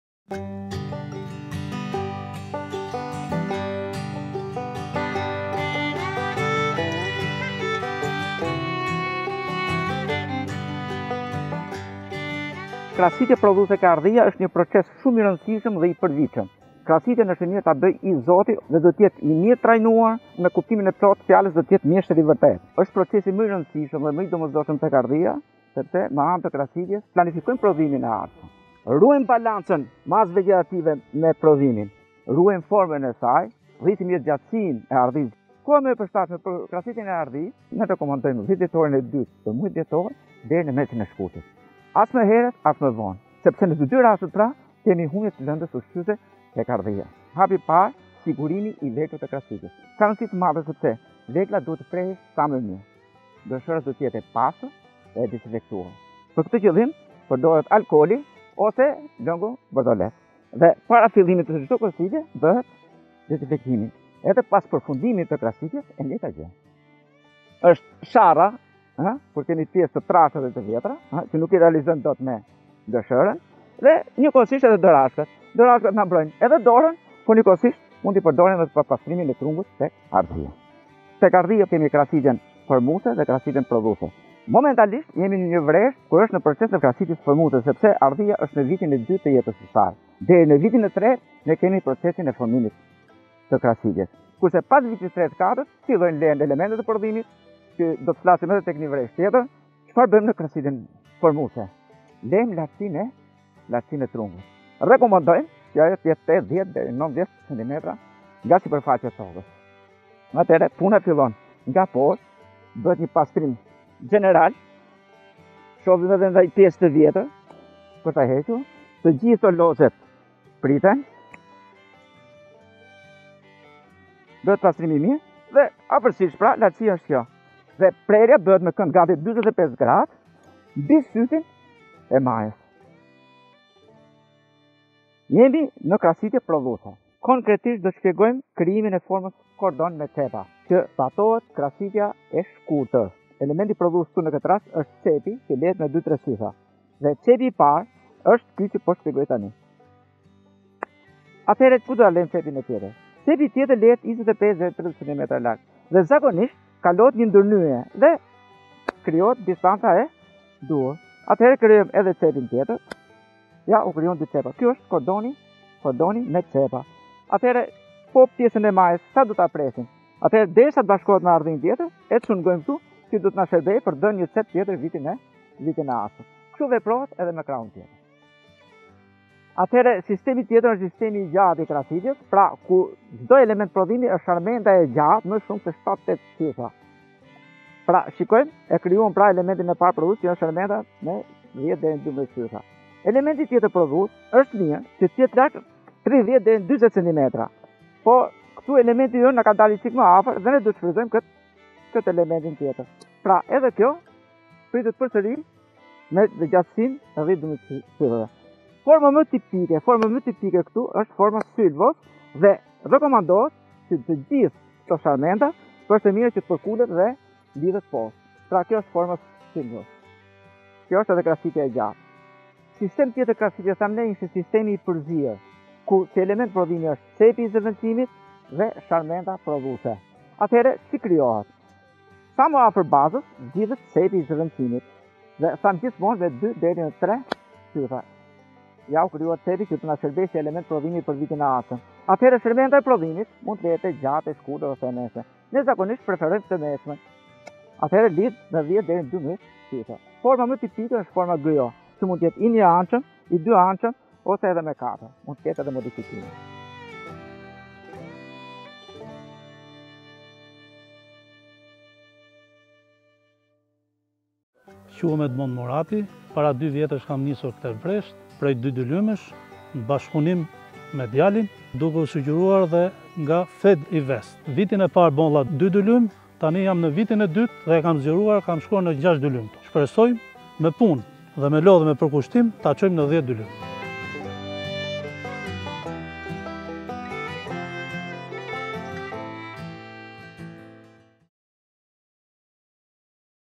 KRASITJA PRODHUESE TEK HARDHIA
Në këtë video paraqiten krasitja formuese, prodhuese dhe rëndësia e këtij procesi tek hardhia. Gjithashtu, trajtohen sistemet e krasitjes, format e mbajtjes së hardhisë, vlerësimi i përgjithshëm dhe ngarkesa që duhet lënë në hardhi, koha më e përshtatshme e krasitjes, si dhe mjetet e domosdoshme për kryerjen e këtij procesi me cilësi dhe shpejtësi.